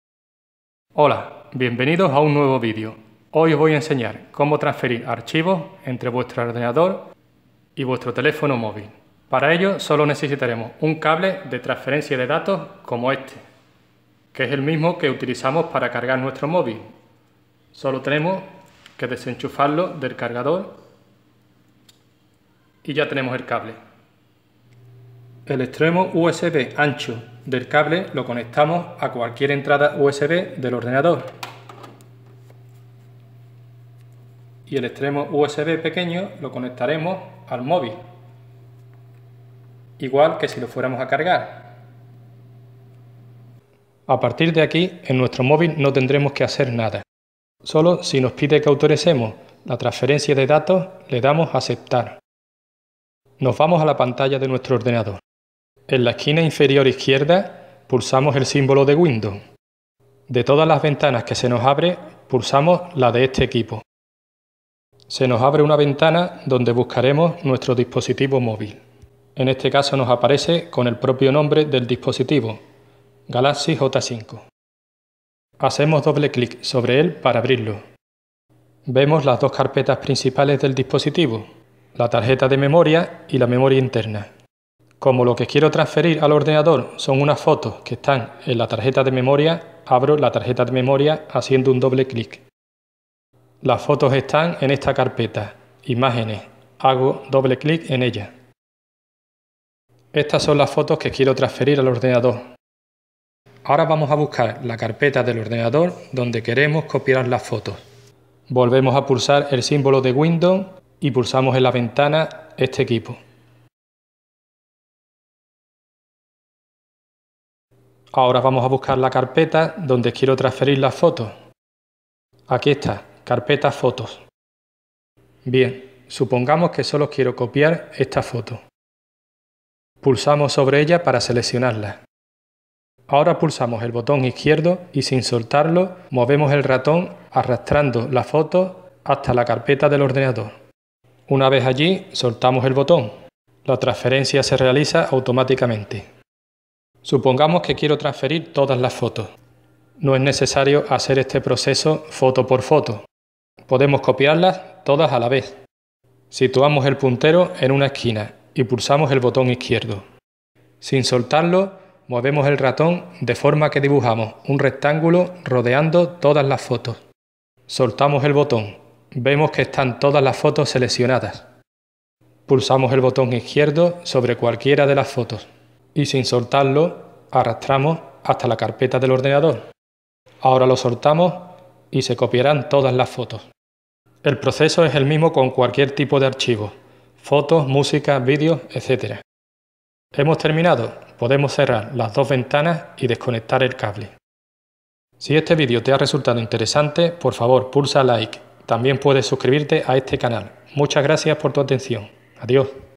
Como PASAR FOTOS Y ARCHIVOS del celular movil al ordenador portátil o PC
Cómo transferir archivos, fotos, vídeos, música entre el teléfono móvil celular y el PC de sobremesa o portatil con cable USB fácil y rápido. En este vídeo te muestro un tutorial de como copiar, mover, pasar y transferir carpetas y archivos entre un ordenador con sistema operativo windows y un telefono movil de cualquier s.o. Android o iOS. Aprende a conectar tu pc a tu celular. Valido para todas las marcas de dispositivos: Samsung galaxy, iPhone, Huawei, Sony, LG, etc.